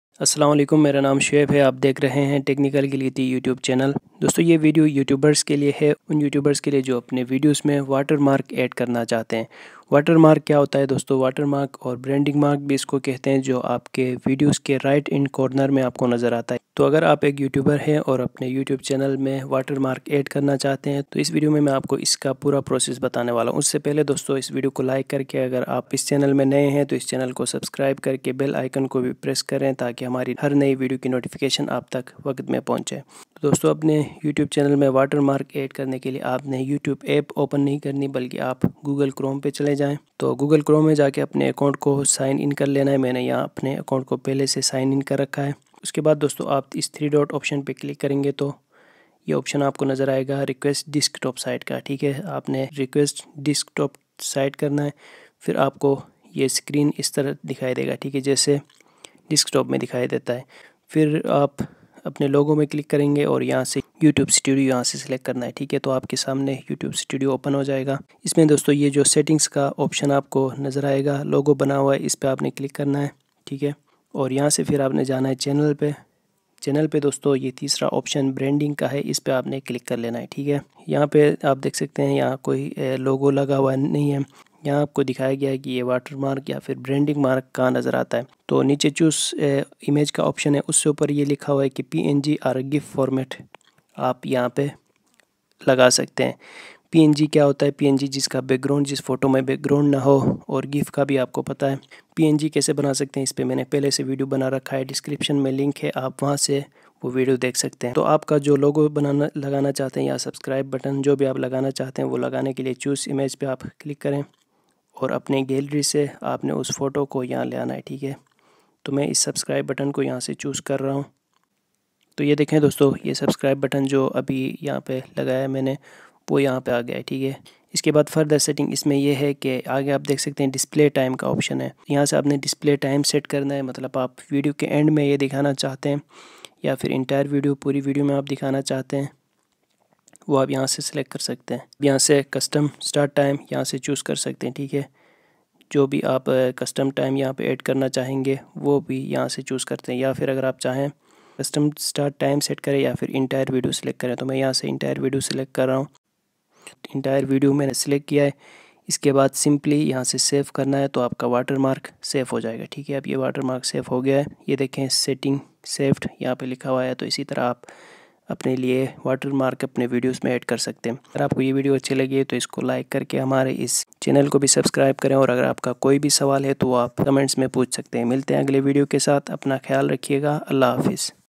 How to Add YouTube Channel Watermark in Urdu
How to Set Youtube Branding | Youtube Studio beta ma Youtube watermark  kaise add kare